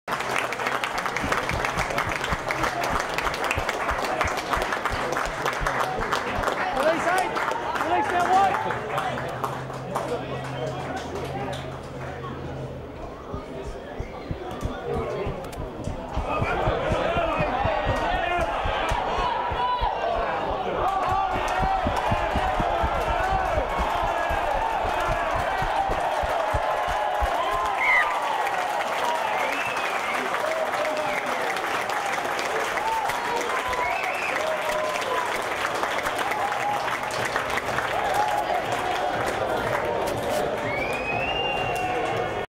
Chilli's '60' metre run to score against Randwick
Chilli runs 40 meters to score